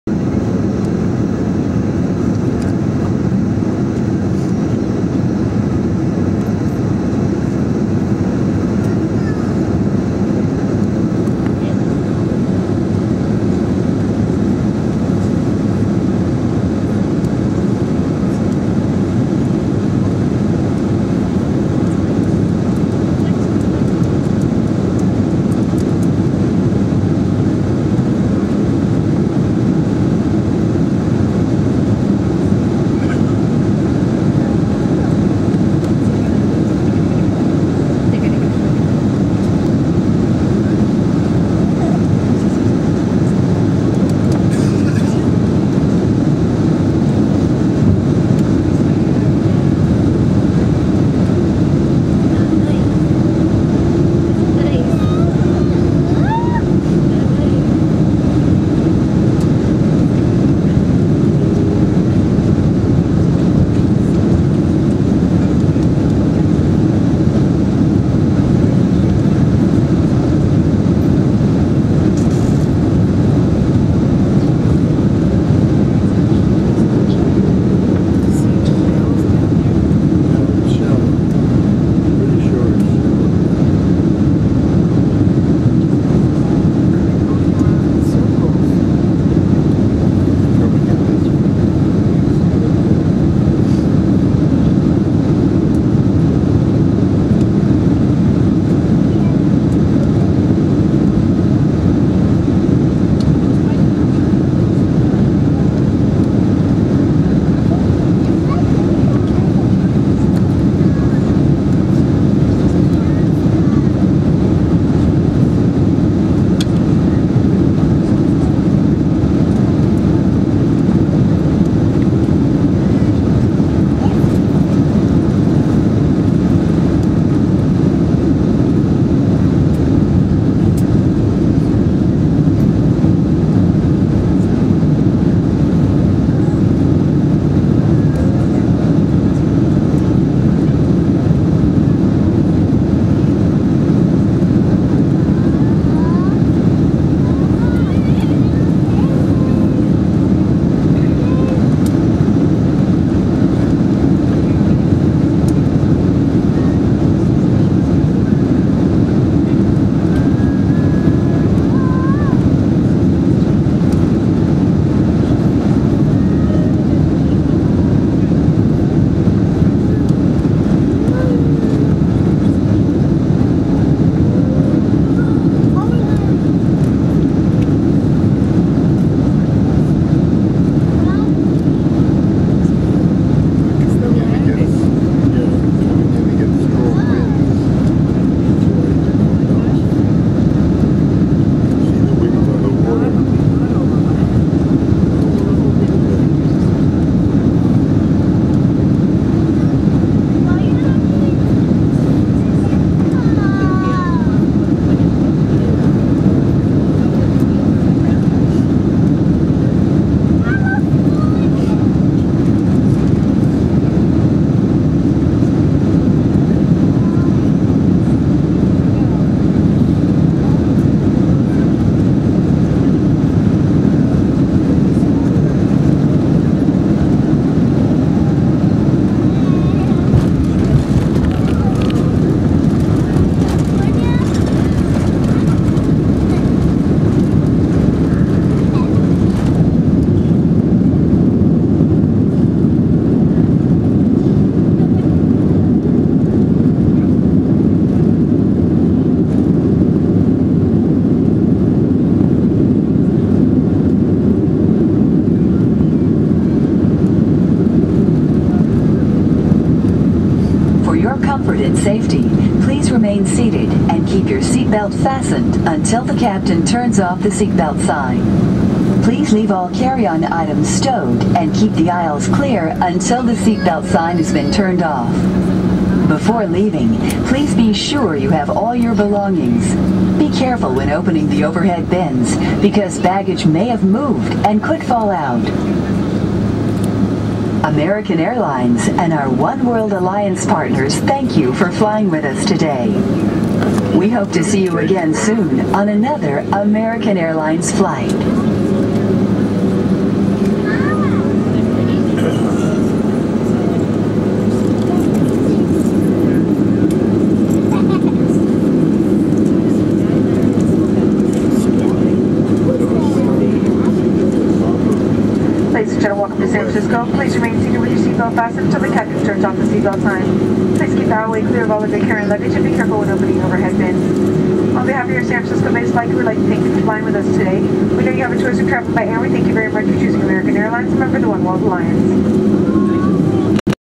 Nice NorCal Sunset: American Airlines 767-300 Landing at SFO
This is the final video of my New York trip. We return landing on runway 28L and you can just catch a glimpse of a Jet Blue A320 departing off of 28R. Gorgeous sunset as well. Hope you enjoy!

Plane: 767-300ER
Time of Day: 7:45pm
Origin: John F. Kennedy International Airport